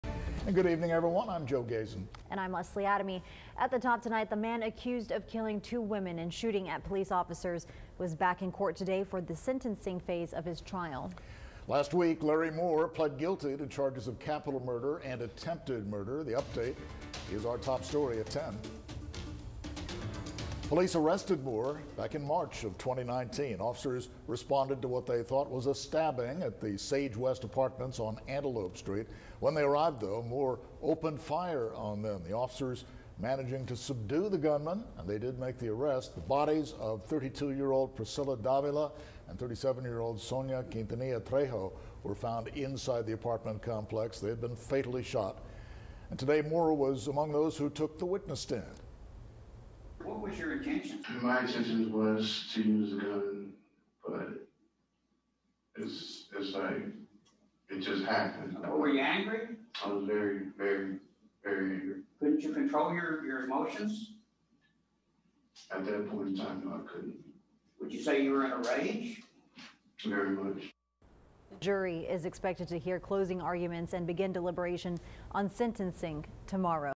Capital Murder suspect takes the stand in Nueces County court
Police arrested Larry Moore in March of 2019 when officers responded to what they thought was a stabbing at the Sage West apartments on Antelope Street.